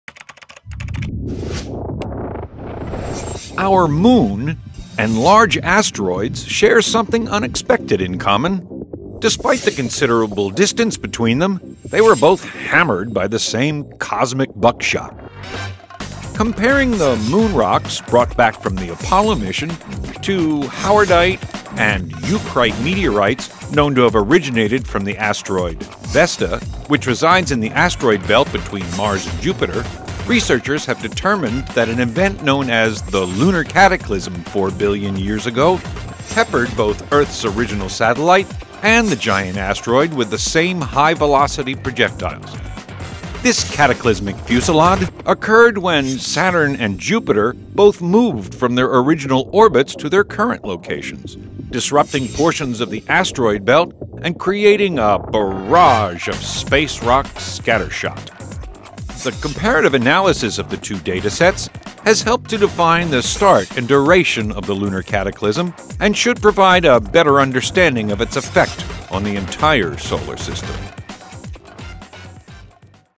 Moon Rocks and Far-Off Asteroids Share Ancestry
Comparing the moon rocks brought back from the Apollo missions to howardite and eucrite meteorites, researchers have determined that an event known as the lunar cataclysm 4 billion years ago peppered both the moon and a far-off asteroid.